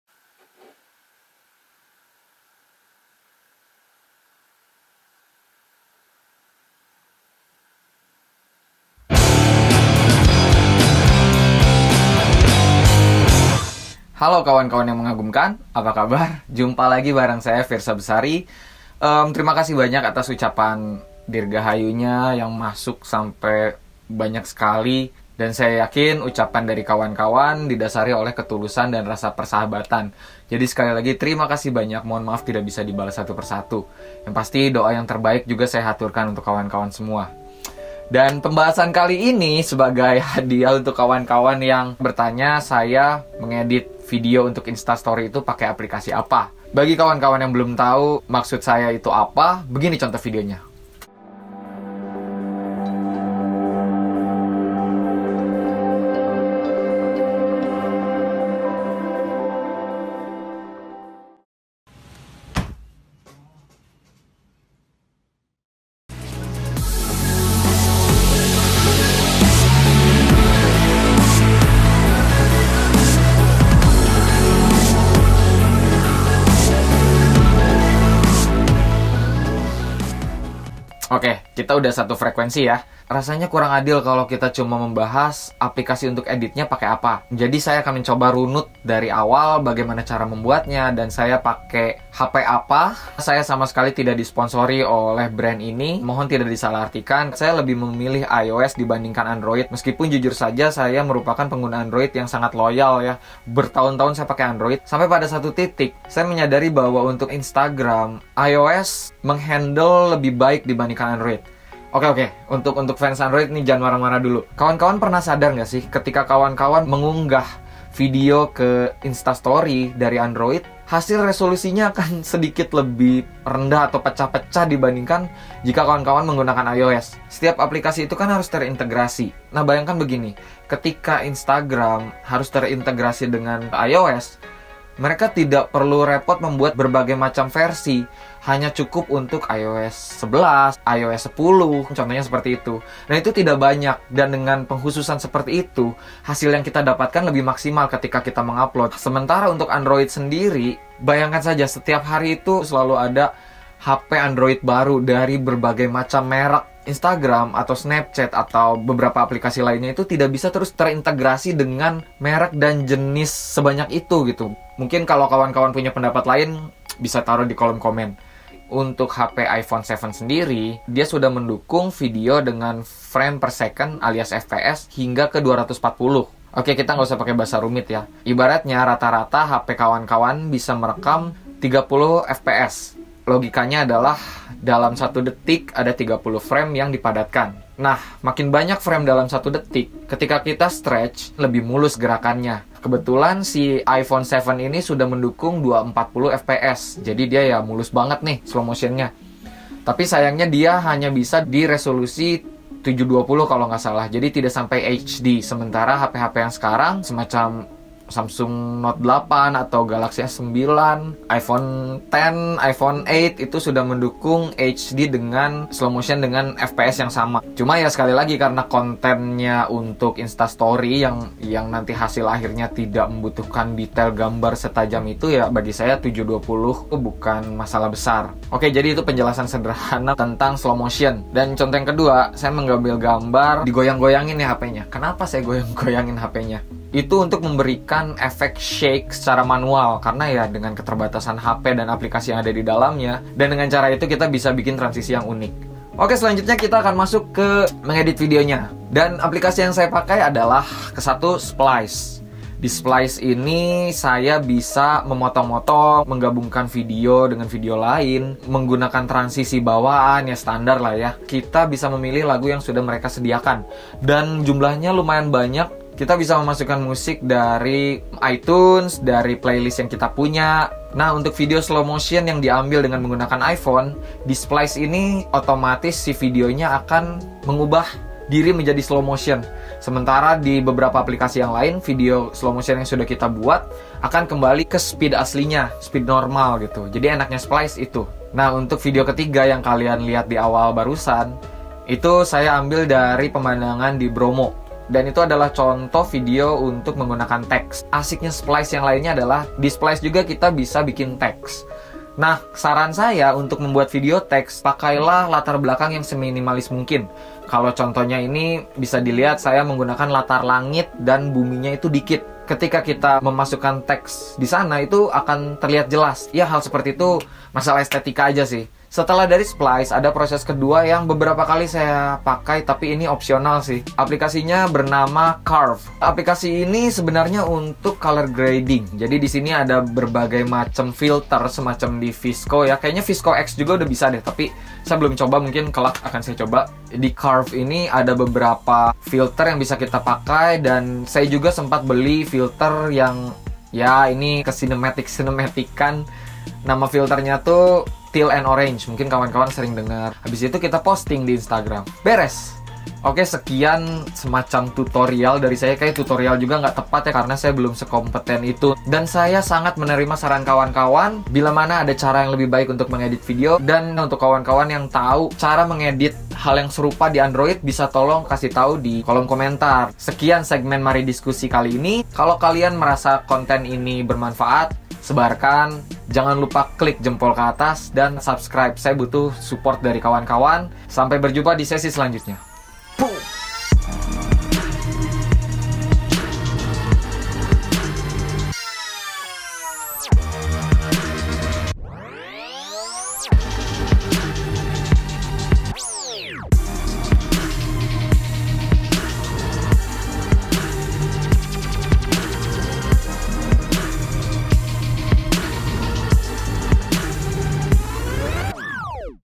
Edit Video Instastory Pakai Apa?
"Mari Diskusi" adalah segmen baru di mana saya akan bercerita tentang apa pun tanpa harus menunggu bertualang. Ini adalah episode kedua, persembahan untuk kalian yang kerap bertanya cara edit video di Instagram. Semoga bisa diterima di hati Kawan-kawan. Uhuy

iOS hanya bisa dipakai di ponsel keluaran Apple. Sementara di Android, tidak sesimpel itu. Ada stock Android (Android murni keluaran Google); ada custom ROM yang biasa dipakai oleh ponsel-ponsel terkemuka semacam Samsung, Xiaomi, Oppo, dsb; ada juga yang terbaru yaitu Android One. Belum lagi jika kita menilik versinya, ada Jellybean, Kitkat, Marshmallow, Nougat, Oreo, dsb. Hal tersebutlah yang membuat beberapa aplikasi tidak terintegrasi dengan baik di Android (semoga kelak akan)

Penjelasan lainnya tentang Instagram versi iOS dan kenapa lebih stabil dibandingkan versi Android